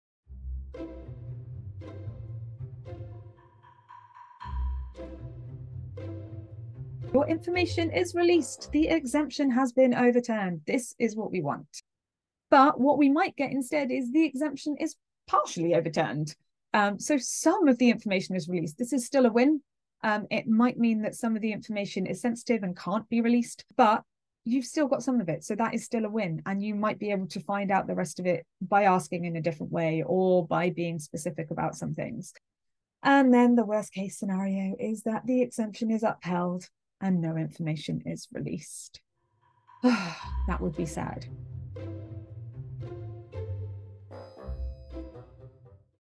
What happens after an Internal Review in FOI?
There are 3 possible outcomes, as explained in this super short video.